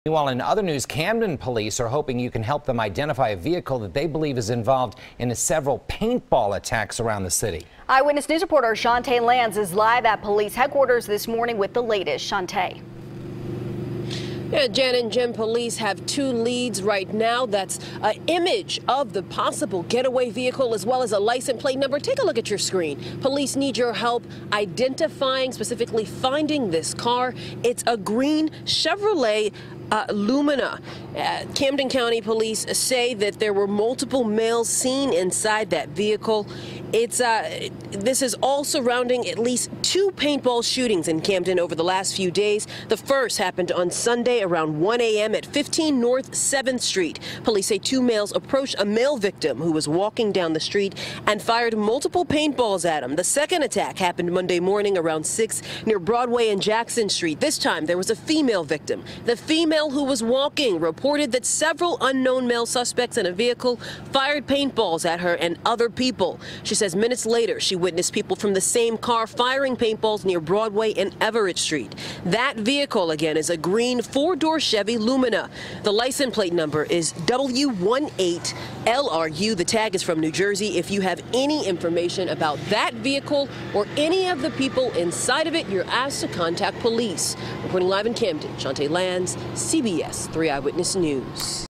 Camden Police Searching For Suspects Involved In 2 Paintball Attacks
Chantee Lans has the latest.